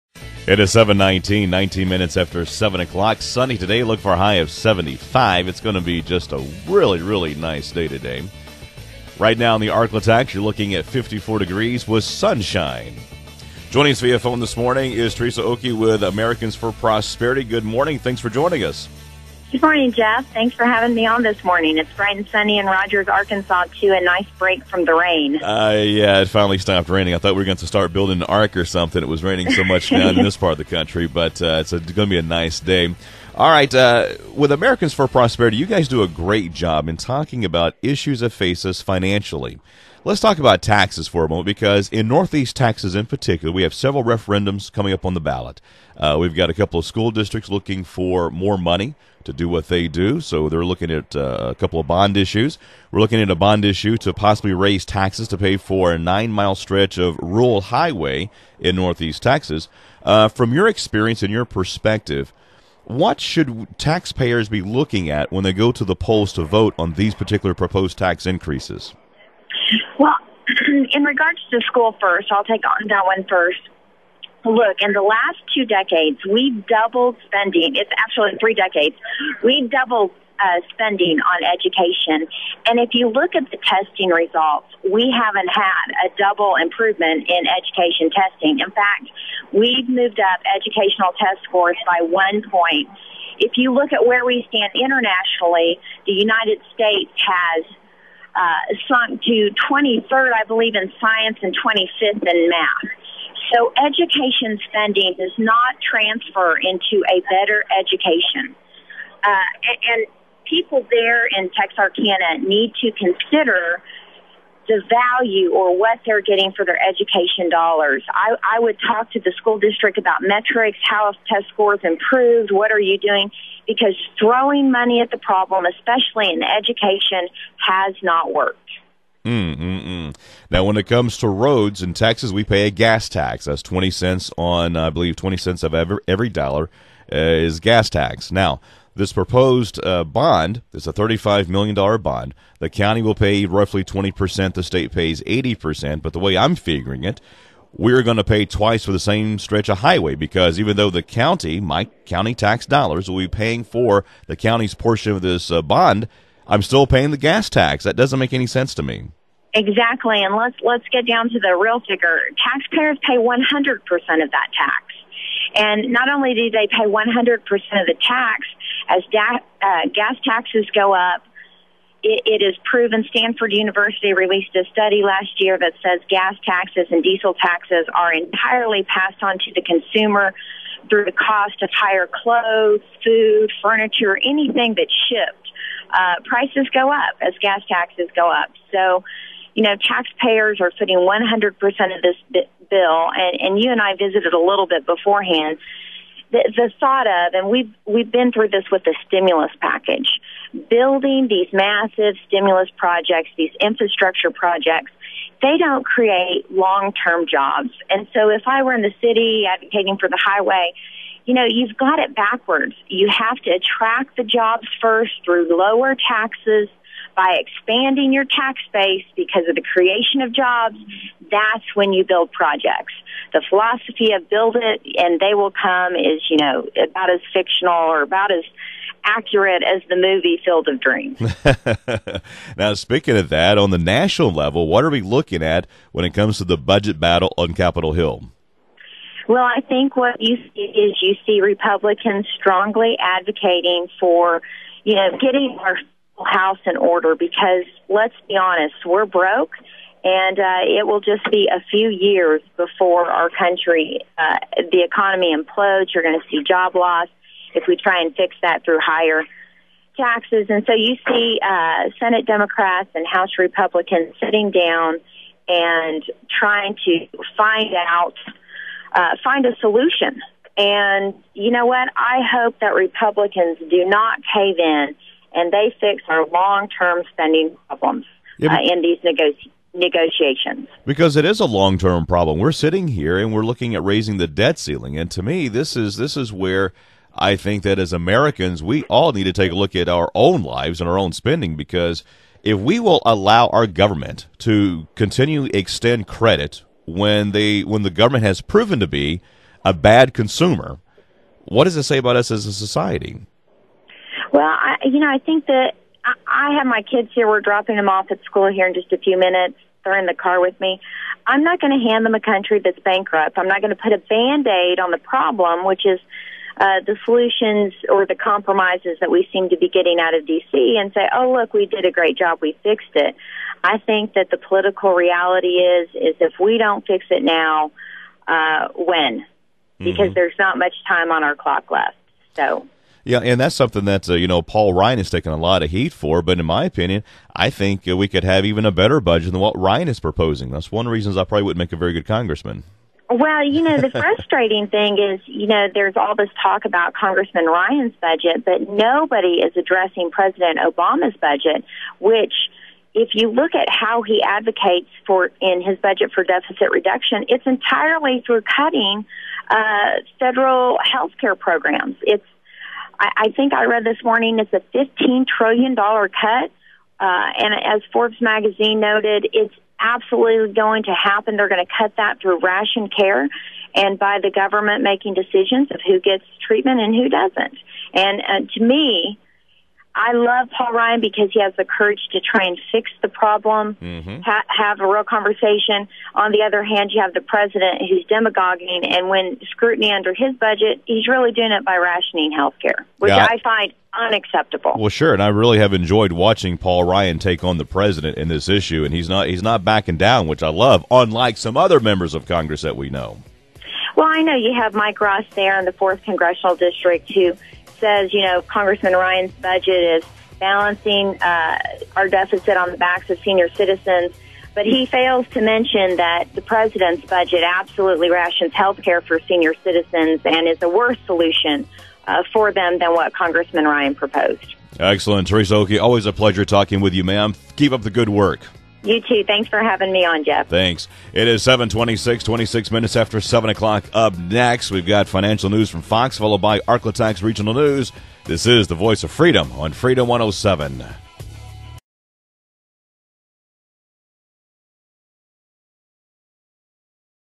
Teresa Oelke Americans for Prosperity.wmv
Teresa Oelke with Americans for Prosperity talked about the proposed tax increases in Northeast Texas.  Voters in Redwater and Daingerfield are being asked to foot the bill for improvements to their schools.  Voters in Bowie County are being asked to pay more taxes for the widening of a stretch of a rural highway.